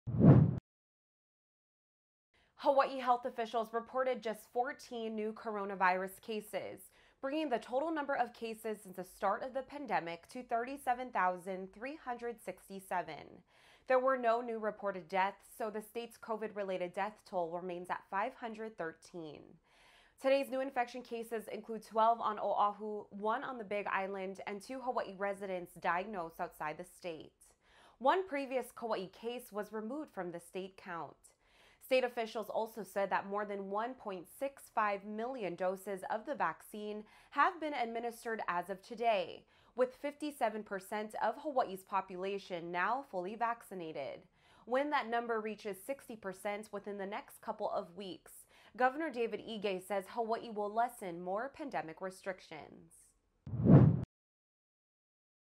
Hawaii Coronavirus Update for 6/21/21
Your Coronavirus update for 6/21/21 with Kiana Cayabyab.